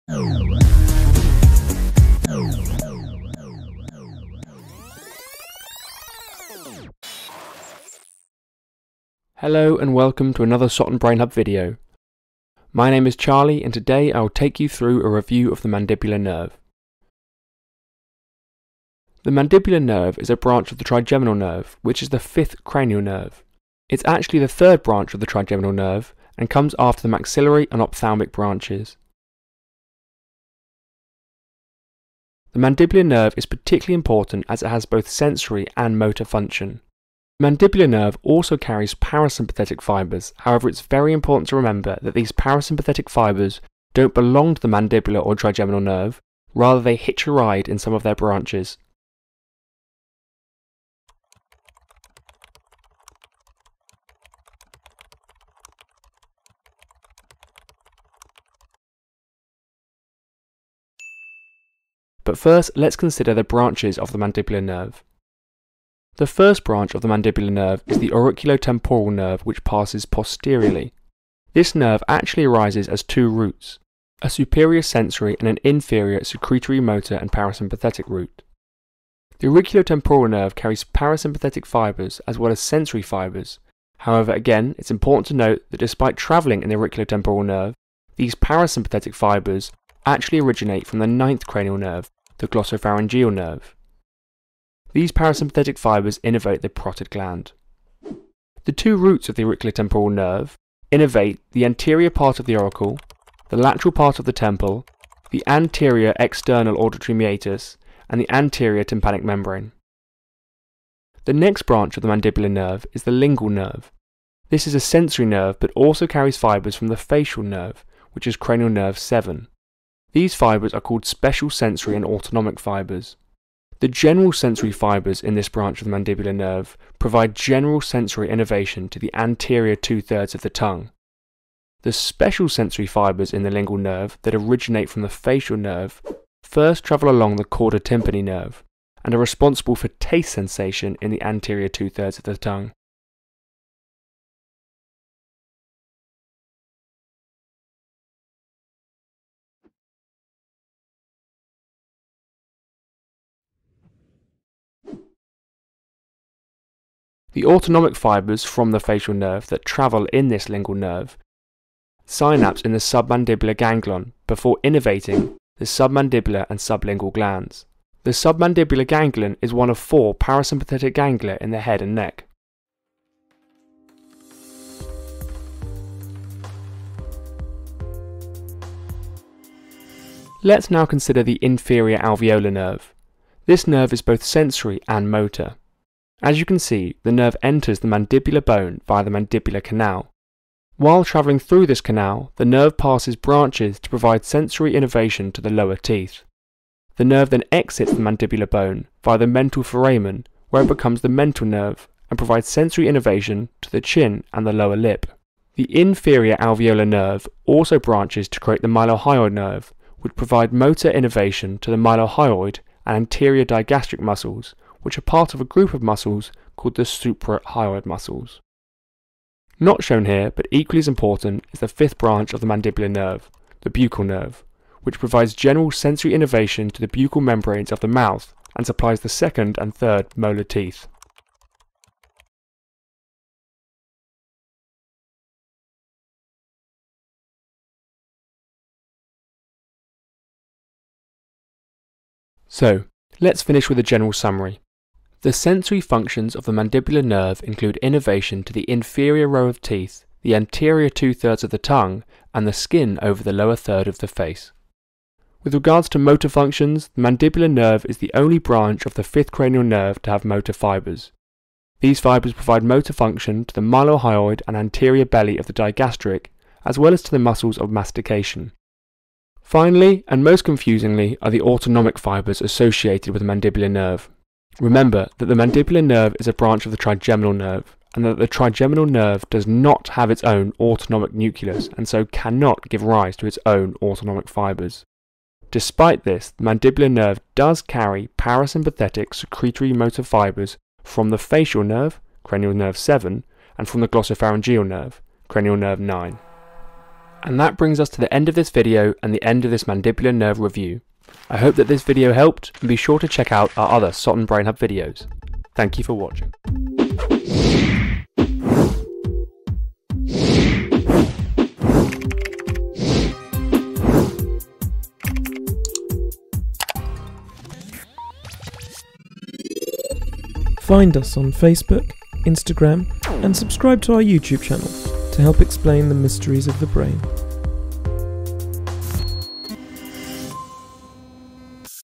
The Mandibular Nerve
This video brings together Charlotte's amazing illustrations from Instagram with Charlie's concise and easy to understand narration, on a common topic in head and neck anatomy. This video covers the essential structures and a simplified description of those tricky autonomic fibres!

for loads of free resources aimed at biomedical and medical students at any stage of training.

Thanks to AE Juice for their super cool motion graphics.

Let us know if there is a particular video you would like to see in our collection and we shall do our best to try and get it covered